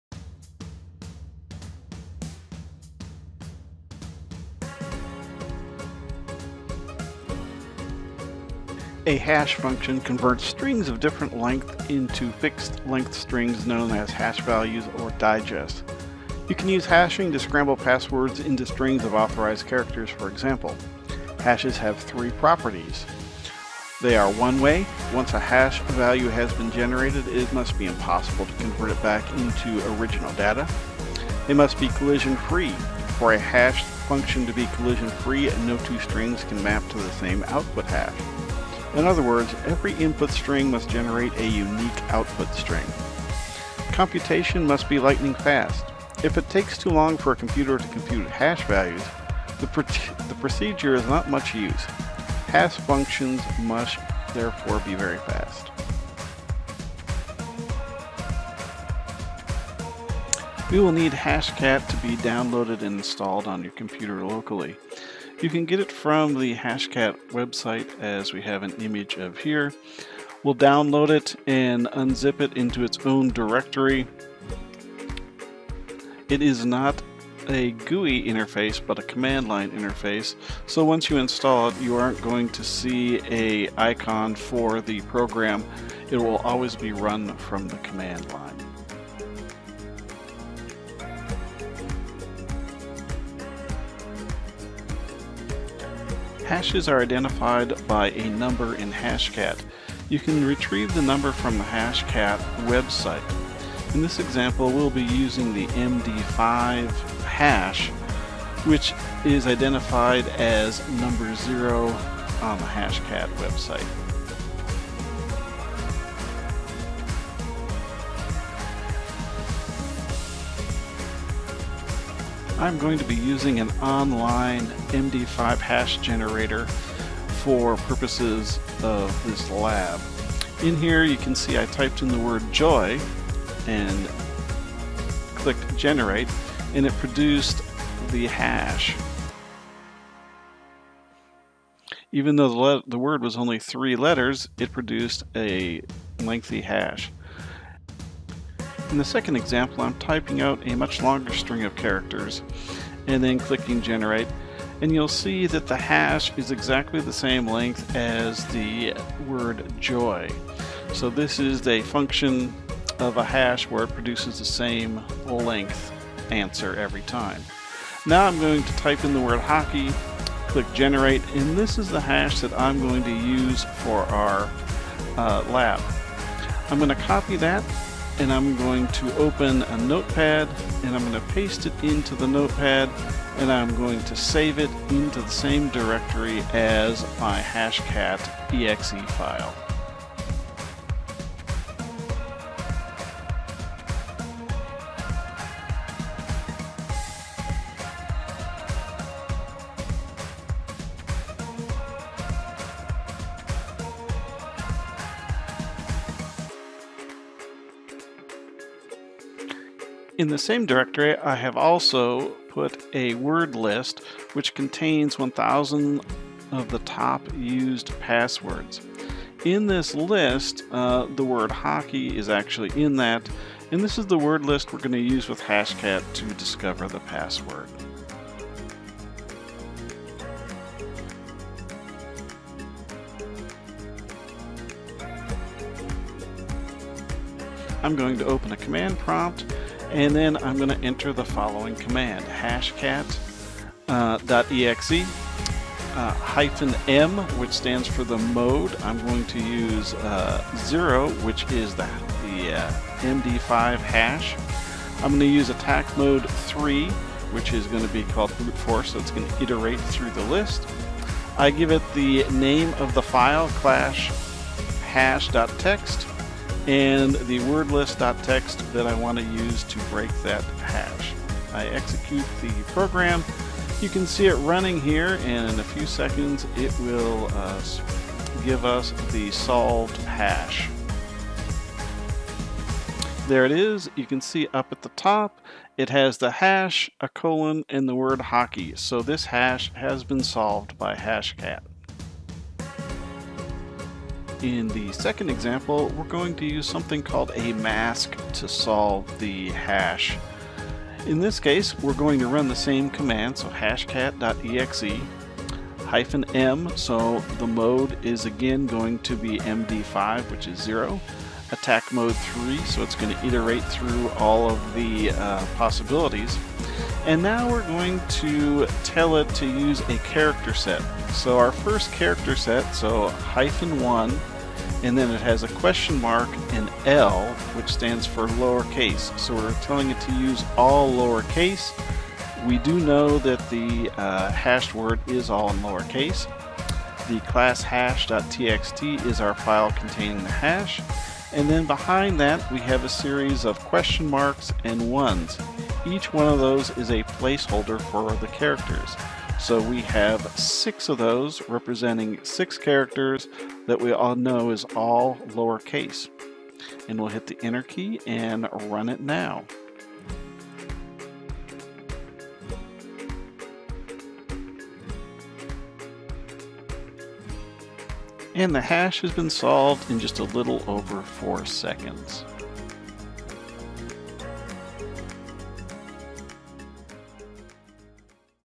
Solving MD5Hashs with hashcat
Using hashcat in Windows 10 to solve MD5 hashes using the wordlist and mask methods.